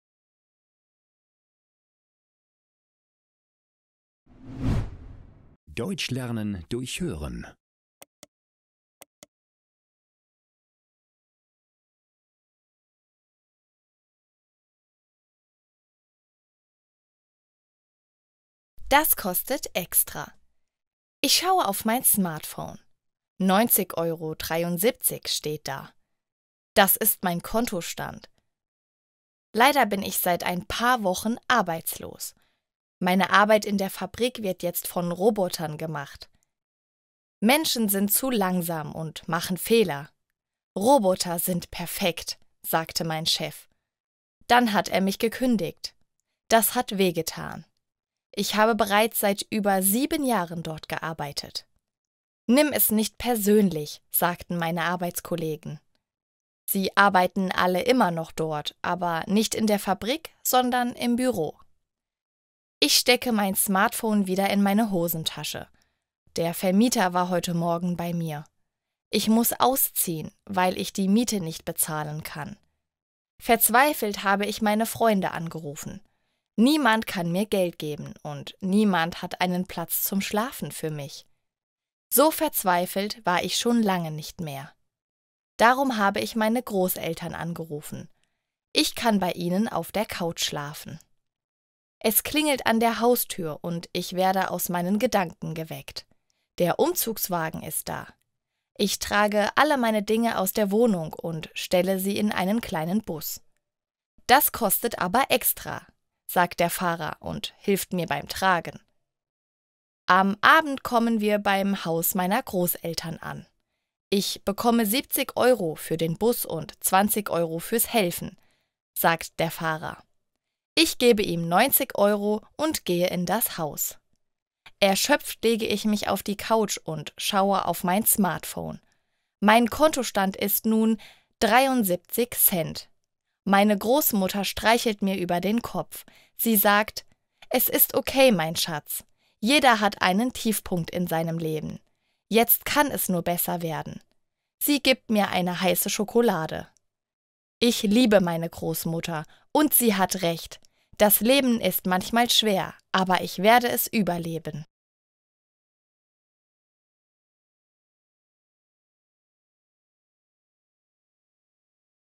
Deutsch für Dich #8 Deutsch lernen mit kurzen Geschichten - Das kostet extra
Unsere App ist verfügbar für IOS und Android! Sie ist kostenlos im App Store und bei Google Play zum Download erhältlich.

𝗛𝗜𝗟𝗙 𝗨𝗡𝗦! ►

Das kostet extra

Ich schaue auf mein Smartphone. 90,73 € steht da. Das ist mein Kontostand.
Leider bin ich seit ein paar Wochen arbeitslos. Meine Arbeit in der Fabrik wird jetzt von Robotern gemacht. „Menschen sind zu langsam und machen Fehler. Roboter sind perfekt“, sagte mein Chef. Dann hat er mich gekündigt. Das hat wehgetan. Ich habe bereits seit über sieben Jahren dort gearbeitet. „Nimm es nicht persönlich“, sagten meine Arbeitskollegen. Sie arbeiten alle immer noch dort, aber nicht in der Fabrik, sondern im Büro.
Ich stecke mein Smartphone wieder in meine Hosentasche. Der Vermieter war heute Morgen bei mir. Ich muss ausziehen, weil ich die Miete nicht bezahlen kann. Verzweifelt habe ich meine Freunde angerufen. Niemand kann mir Geld geben und niemand hat einen Platz zum Schlafen für mich. So verzweifelt war ich schon lange nicht mehr. Darum habe ich meine Großeltern angerufen. Ich kann bei ihnen auf der Couch schlafen.
Es klingelt an der Haustür und ich werde aus meinen Gedanken geweckt. Der Umzugswagen ist da. Ich trage alle meine Dinge aus der Wohnung und stelle sie in den kleinen Bus. „Das kostet aber extra“, sagt der Fahrer und hilft mir beim Tragen.
Am Abend kommen wir beim Haus meiner Großeltern an. „Ich bekomme 70 Euro für den Bus und 20 Euro fürs Helfen“, sagt der Fahrer. Ich gebe ihm 90 Euro und gehe in das Haus.
Erschöpft lege ich mich auf die Couch und schaue auf mein Smartphone. Mein Kontostand ist nun 0,73 €. Meine Großmutter streichelt mir über den Kopf. Sie sagt: „Es ist okay, mein Schatz. Jeder hat einen Tiefpunkt in seinem Leben. Jetzt kann es nur besser werden.“ Sie gibt mir eine heiße Schokolade. Ich liebe meine Großmutter und sie hat recht. Das Leben ist manchmal schwer, aber ich werde es überleben.